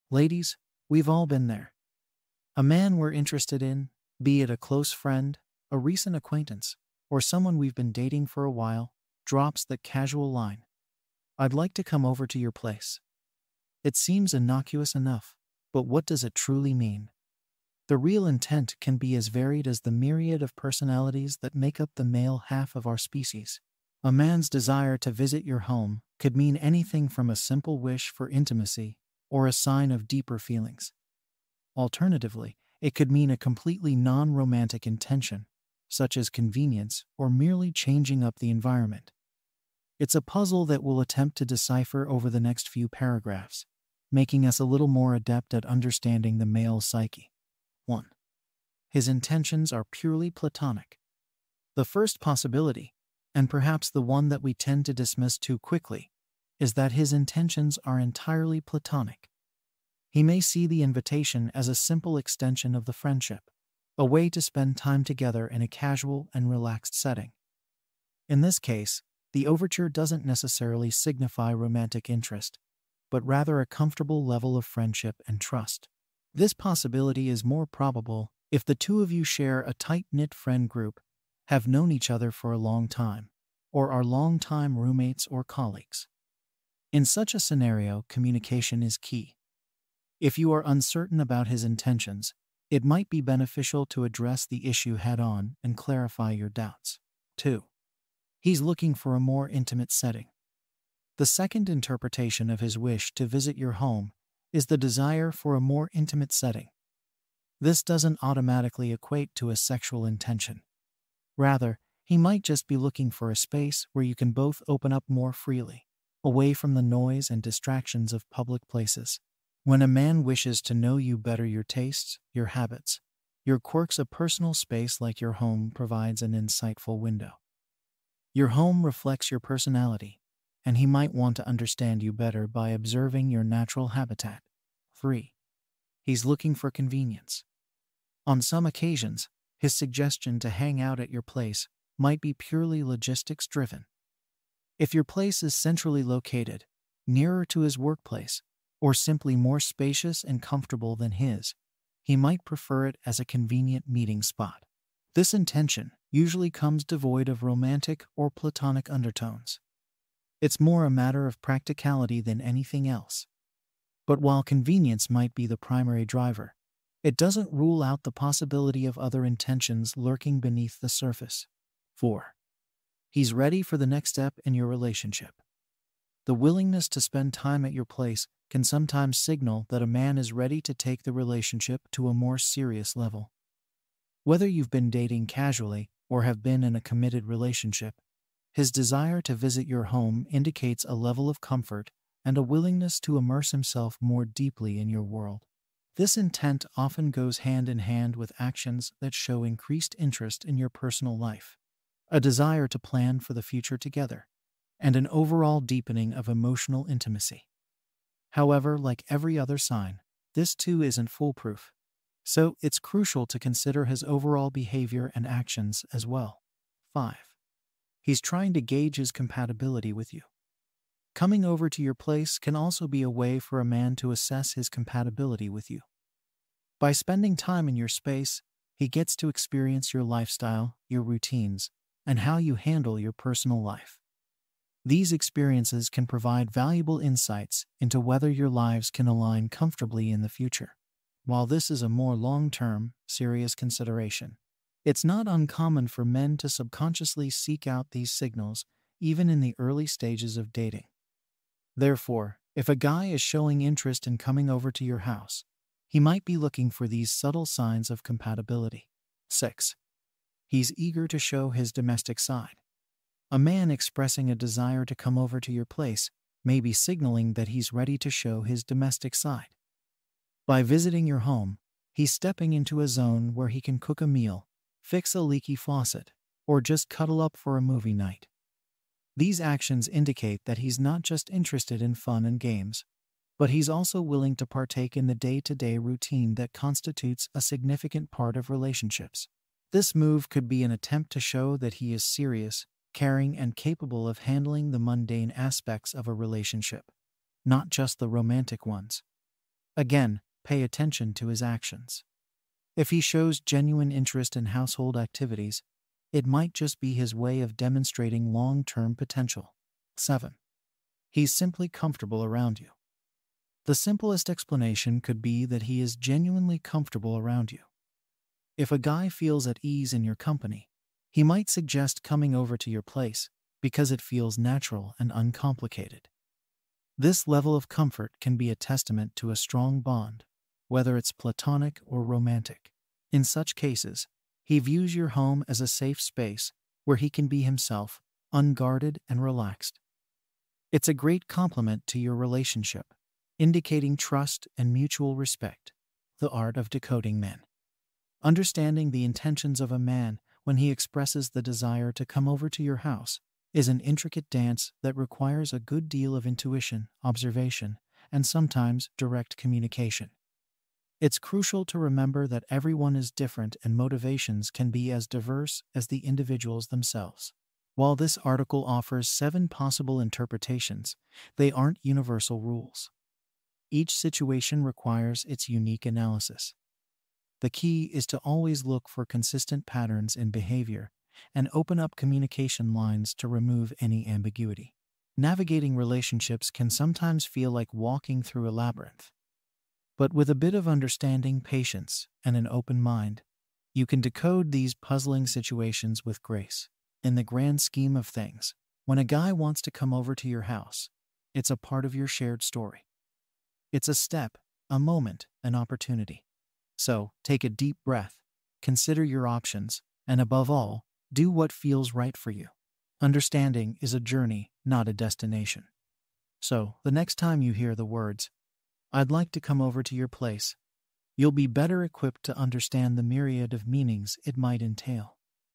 7 Reasons: When a Guy Wants to Come Over Your House
Dive deep into understanding the meaning behind the statement 'When a guy wants to come over your house'. This comprehensive guide provides seven surprising insights that reveal the motives and expectations that might be hidden behind this seemingly casual suggestion.